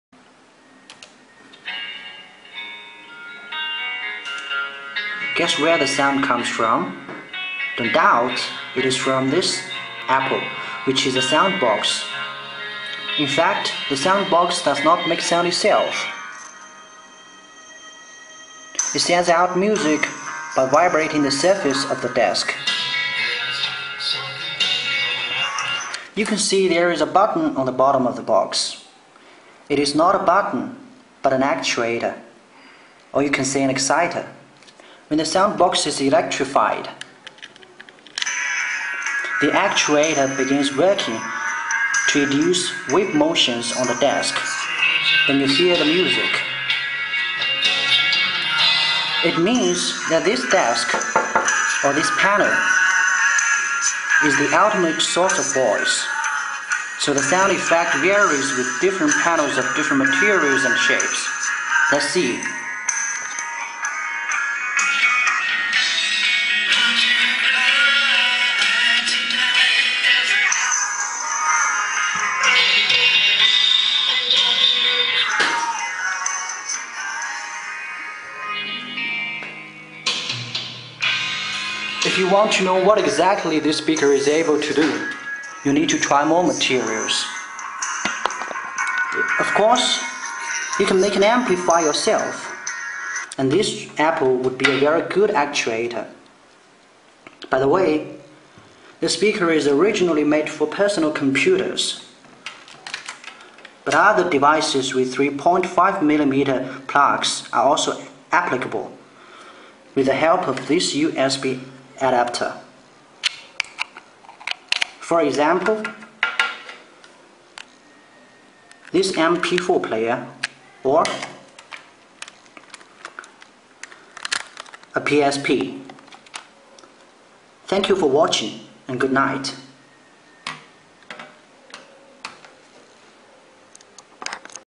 USB Vibrating Audio Resonance Speaker
Mini USB Powered Vibrating Audio Resonance Speaker Singing Table Surface Apple Design for PC
Music that vibrates through your desk!
Non-traditional speaker design, creates resonance on surface
Vibration of the music penetrates through your desk or tabletop